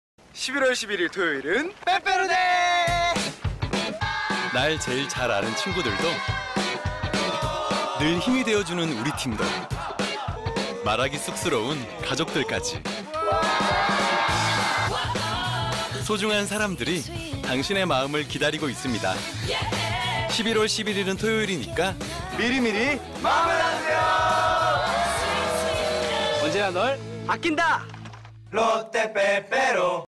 2017년 롯데 빼빼로 CF (미리미리 마음을 나누세요 편 - EXO-K 출연)
보이 그룹 엑소(EXO)의 유닛인 EXO-K의 멤버들이 주변 사람들에게 빼빼로를 나눠주는 모습을 통해 "11월 11일 빼빼로데이에는 미리미리 마음을 나누자"는 메시지를 전달한 CF.